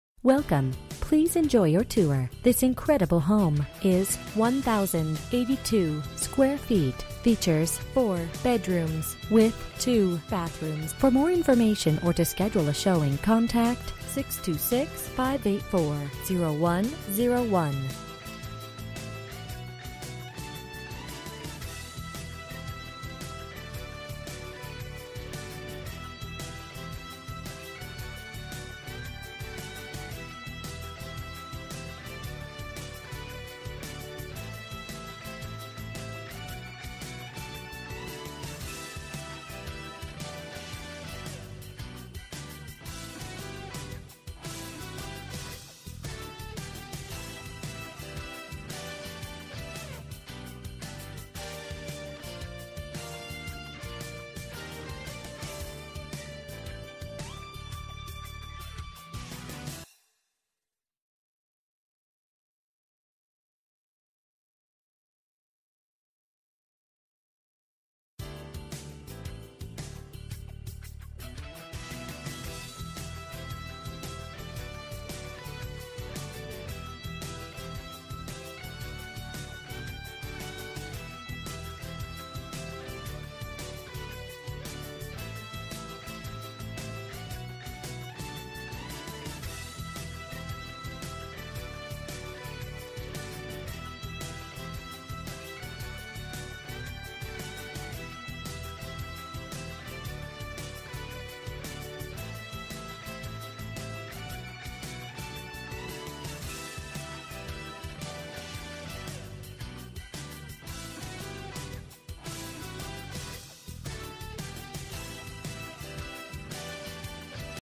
Residential for sale - 3435 MERCED Street, Los Angeles, CA 90065
Wow! Quality remodel from top to bottom! New plumbing systems, HVAC, dual pane windows &amp; doors, can lighting, kitchen &amp; bathrooms, flooring &amp; flooring supports, roofing, etc. This 1920 house has entered the 21st century and is ready for move in! Room to park multiple cars, generous flat backyard with fruit trees and covered patio. There is a detached 360sqft bonus room for art studio, gym, or whatever suits your fancy. Master suite has a walk-in closet, glass enclosure for shower, and sliding patio door. Kitchen has separate laundry closet for your convenience and all new kitchen appliances. Located in a friendly up &amp; coming neighborhood in Cypress Park. Like a new home, so why not make it your welcome home!     Updated/Remodeled

Bedrooms: 4
Bathrooms: 2
Square Feet: 1082
Price: $579,000

08/23/2018 04:02:02 am

Last modified: